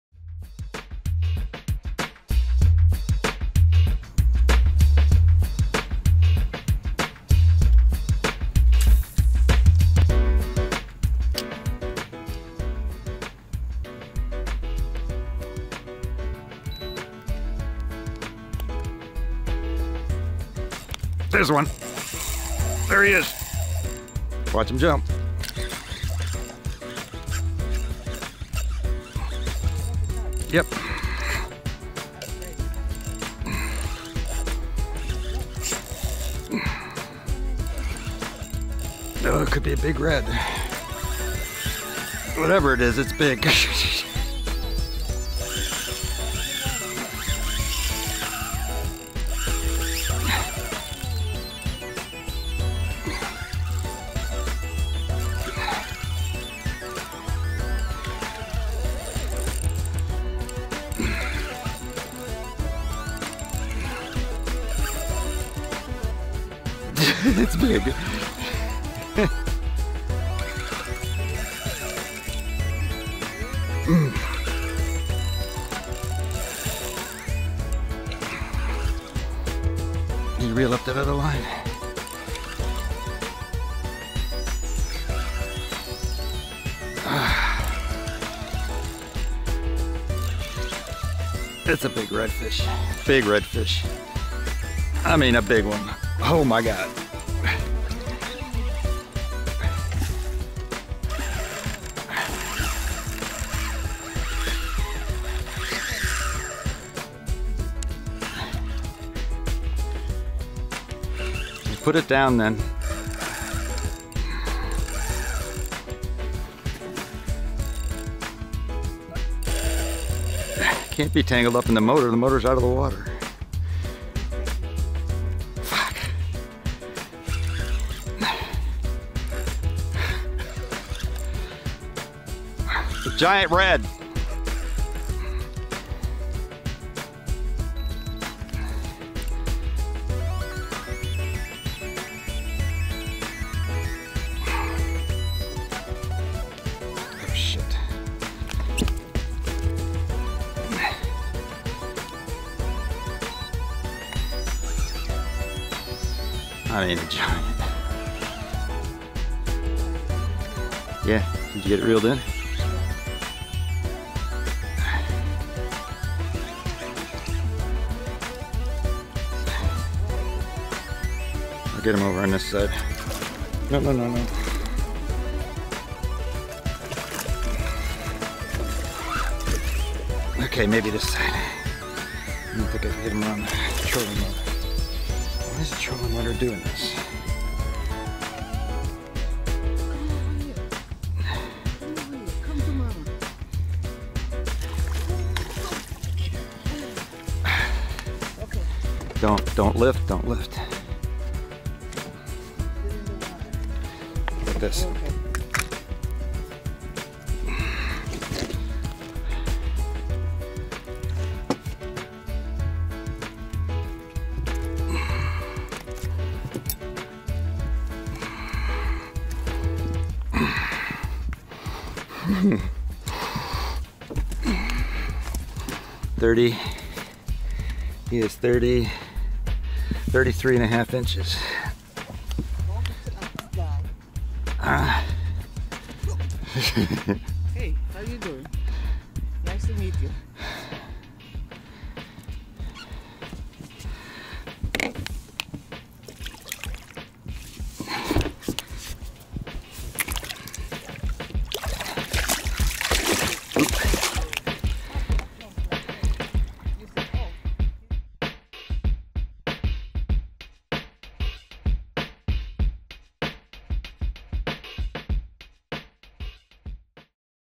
Giant Redfish, Calm Water Morning, Orange Park Florida
Giant Redfish, Calm Water Morning, St Johns River, Orange Park, Florida

Caught on Academy Sports H2O Crazy Croaker swim bait.